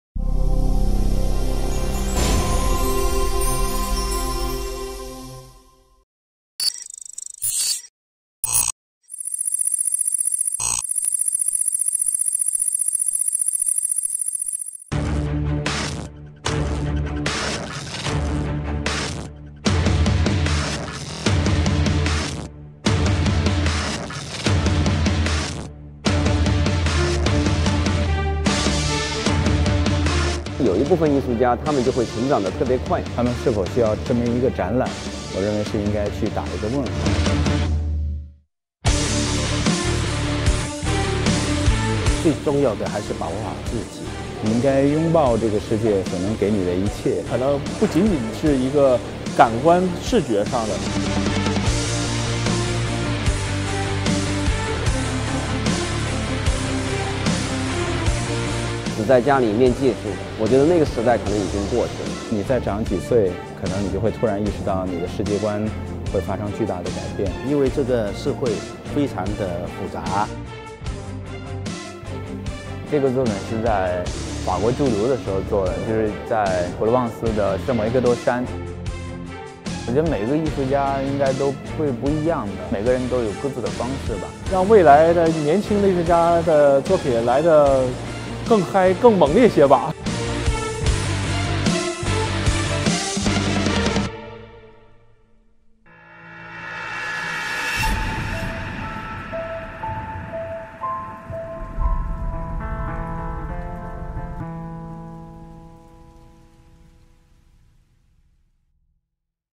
【ActionMedia】 Exhibition : The 2nd "CAFAM · Future" Exhibition Observer - Creator | CAFA Art Museum
The Reality Representation of Chinese Young Art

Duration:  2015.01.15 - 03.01
Venue: CAFA Art Museum, 798 Art Zone

Chief Curators: Xu Bing, Jean de Loisy
Art Director: Wang Huangsheng
Artistic Adviser: Wang Chuncheng, Sheng Wei, Li Zhenhua, Tsai Meng
Assistant Curators: Gao Gao, Liu Xiyan
Exhibition Coordinators: Fu Dandan, Song Liang